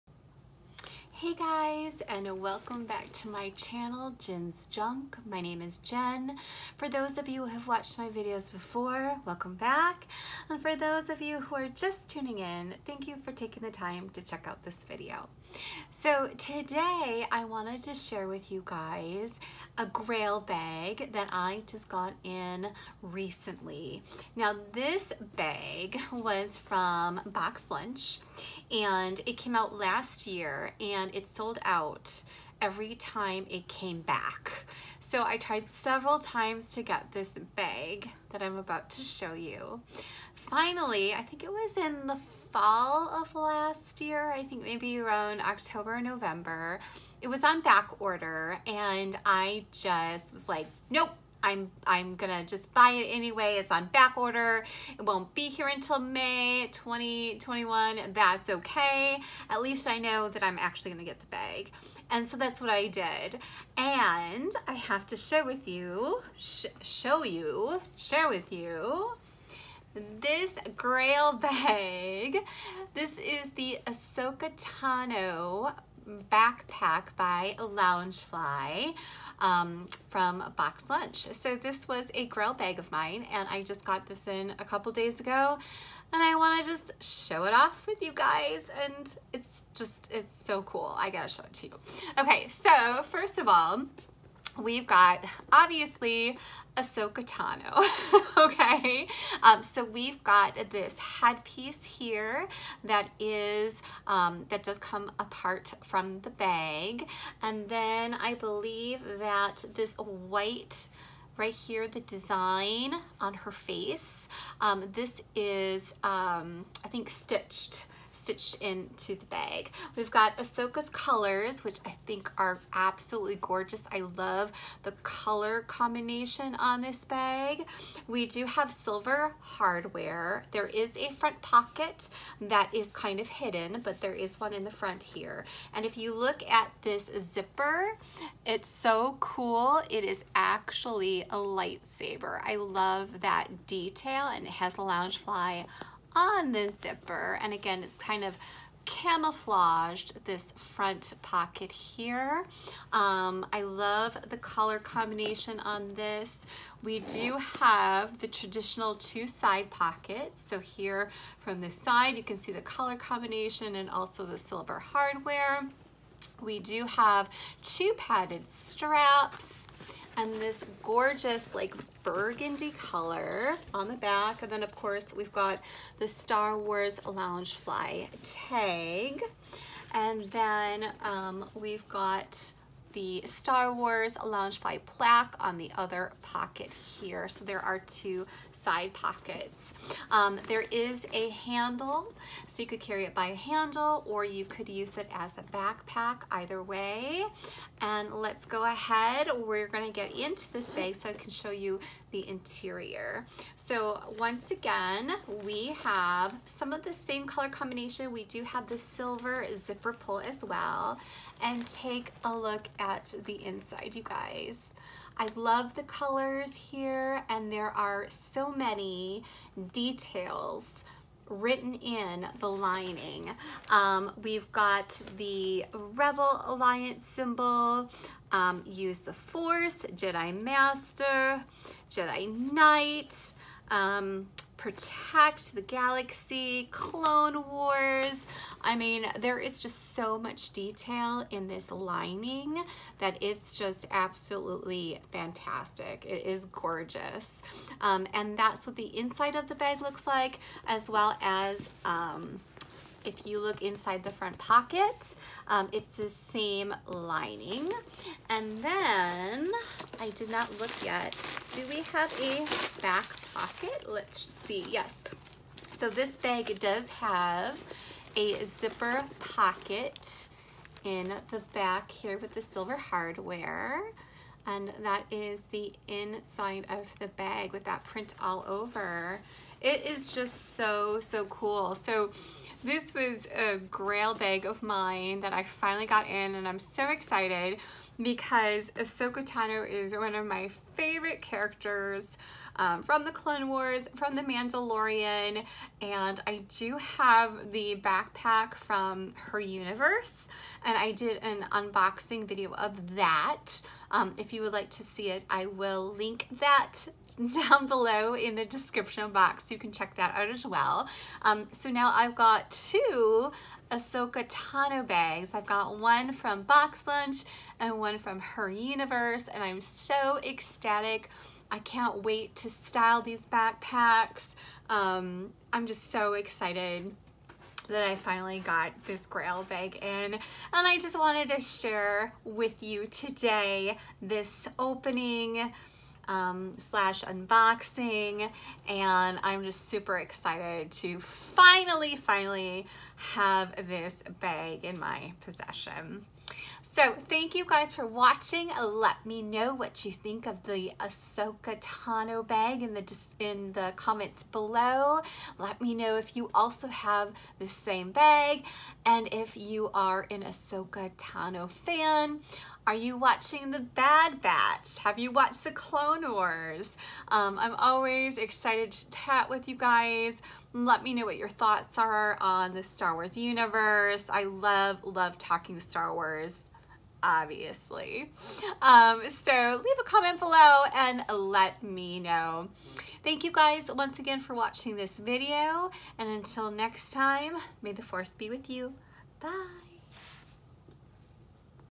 Ahsoka Tano Boxlunch Backpack Unboxing
In today's video, I show one of my Grail bags from Box Lunch!